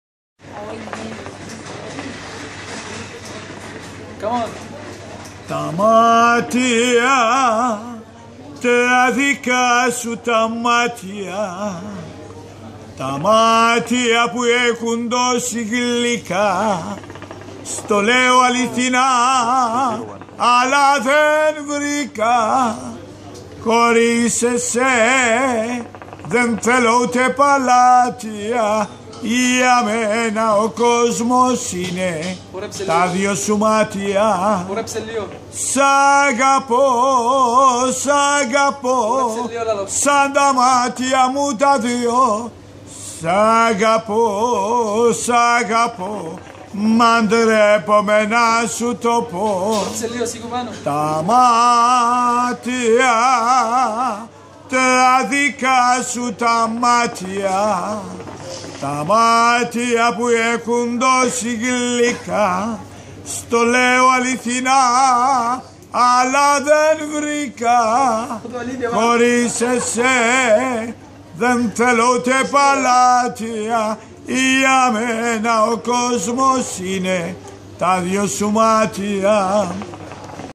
Giannakis Tsaggarides
76 years old guy singing